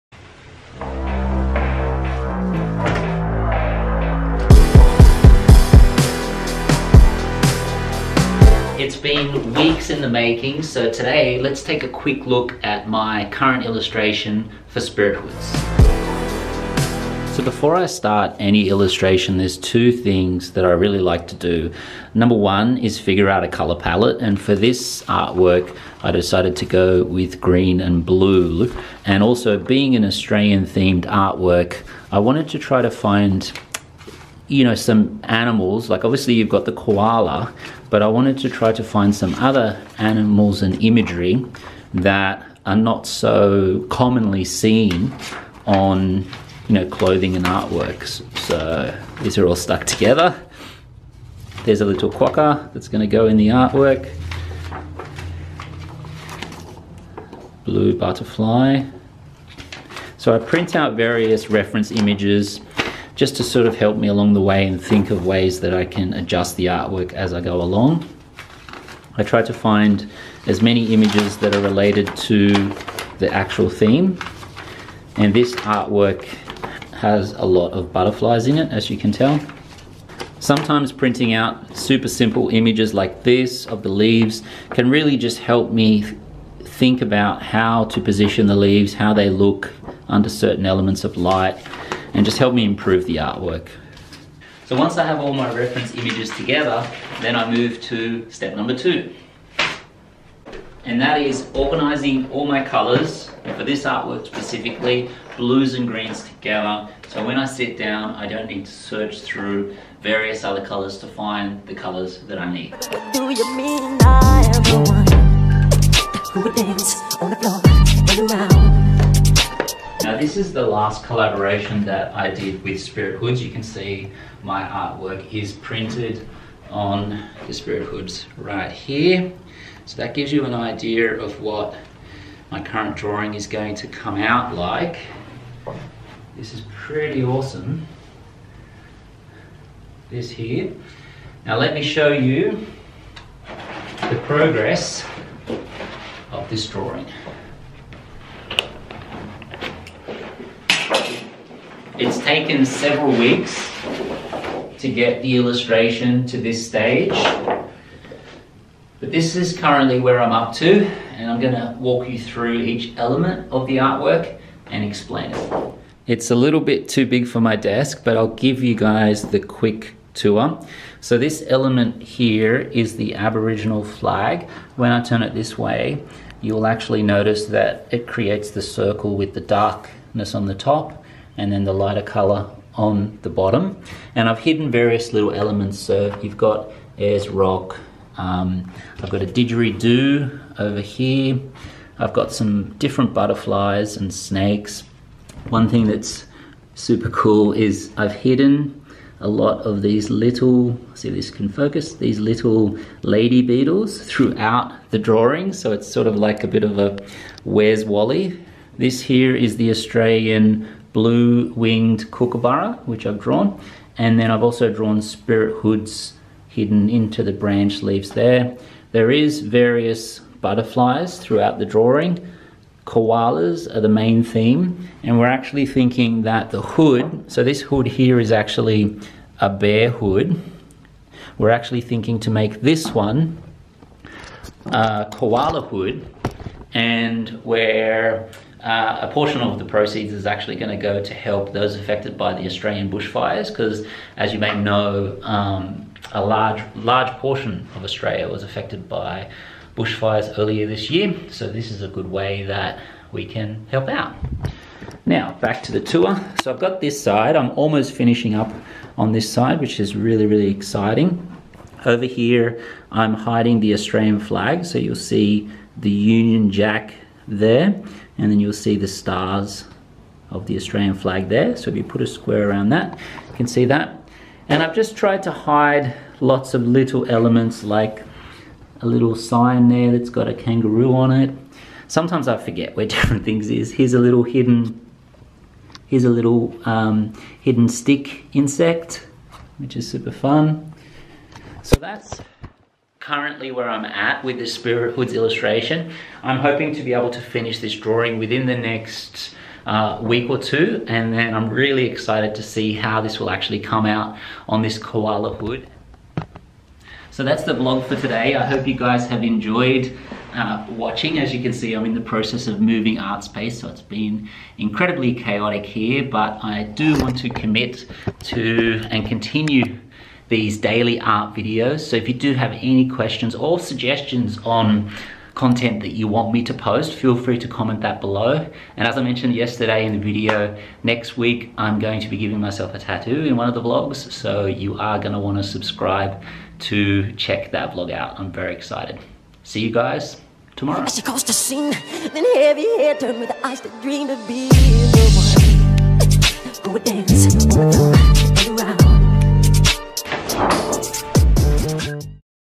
Koala Spirithoods drawing update
Close to finishing my Artwork for Spirithoods, I thought in todays vlog we could take a look at the drawing, it's meaning.. and how it'll help the Koala.